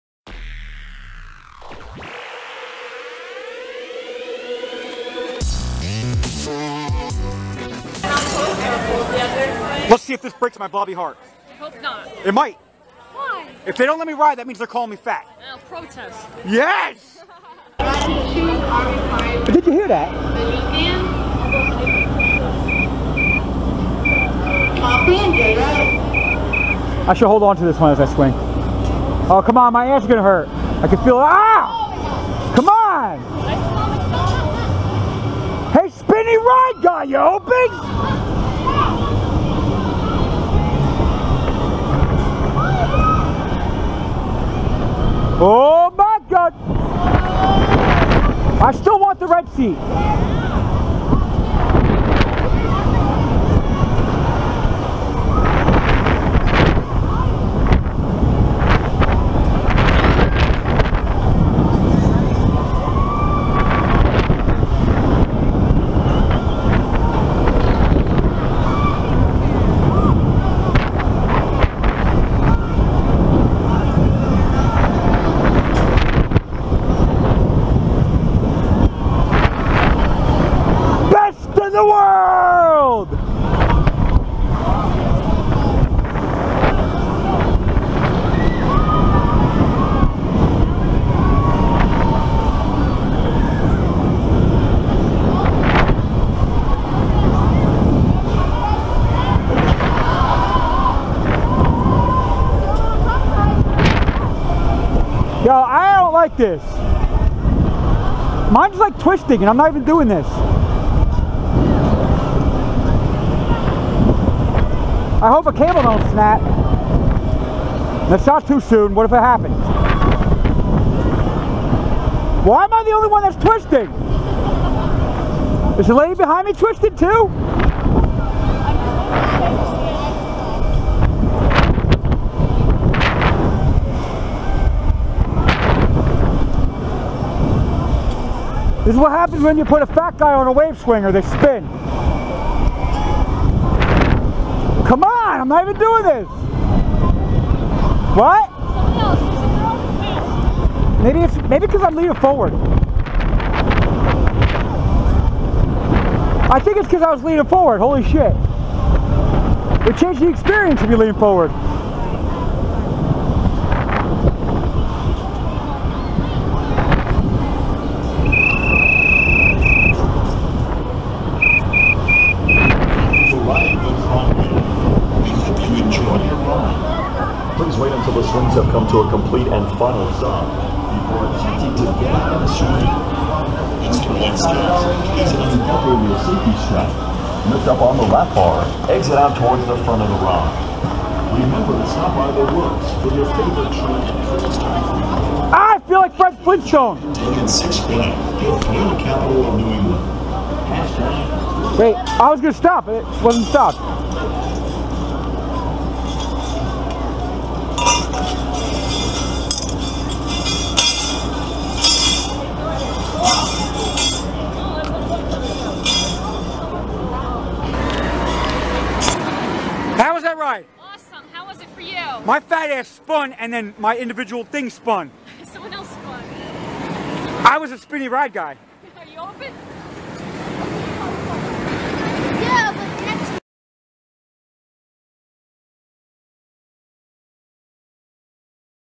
Six Flags New England: Gotham City Crime Wave / On Ride POV / August 21, 2014 / 1080P
The Gotham City Crime Wave is Six Flags New England's version of the Waveswinger! Check out the beauty of this ride as it has a Batman theme to it!